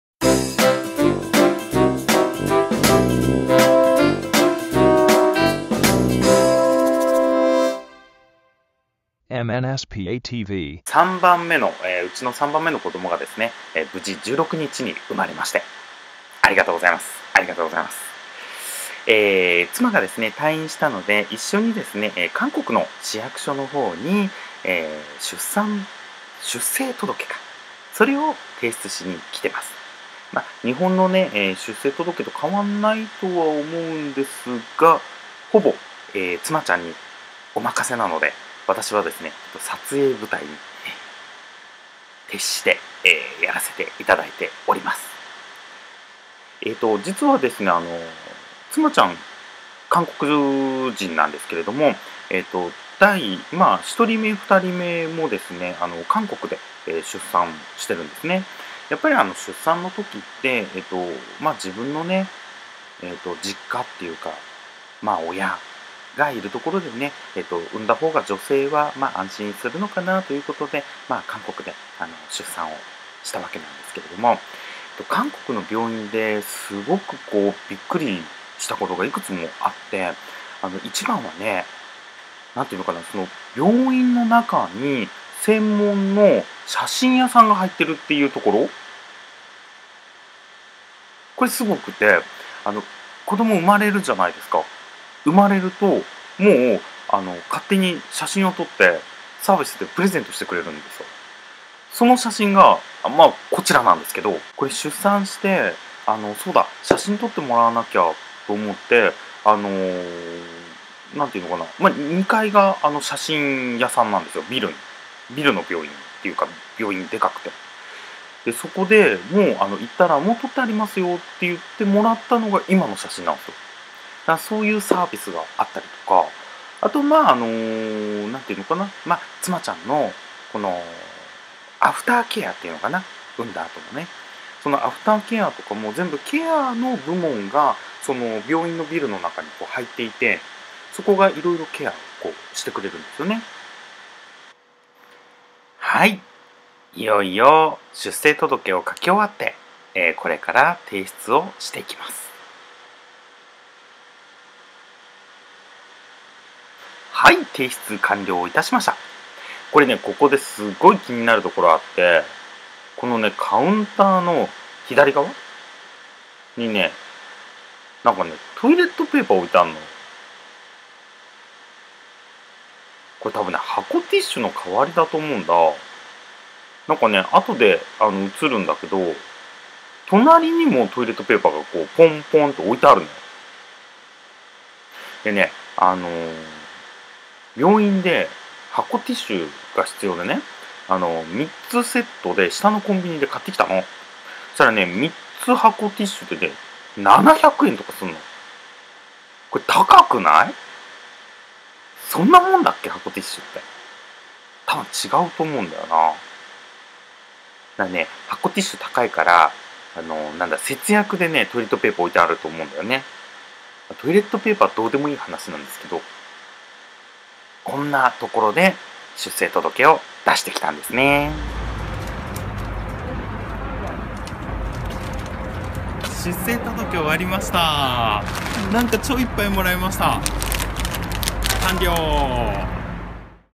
出産後の手続きは意外と多い！まずはとにかく出生届け出して来たぞ〜！in韓国なのだ
※今回は、韓国釜山で第３子を出産してまいりました。妻がね？

出産後の手続きは意外と多い！

①出生届 出産日を含め14日以内
②健康保険の加入 1カ月検診まで
③乳幼児医療費助成 子供の健康保険加入後、1カ月検診まで
④児童手当金 出生してから15日以内
⑤出産育児一時金・付加金 出産した翌日から2年間
⑥出産手当金 産後56日以降
⑦育児休業給付金 育児休業1カ月前まで
⑧高額医療費 診察日の翌月から2年以内

とにもかくにも、まずは、出産届け！！

韓国釜山で役所に出産届を出して来たよ！っということでどうぞ、ご覧ください。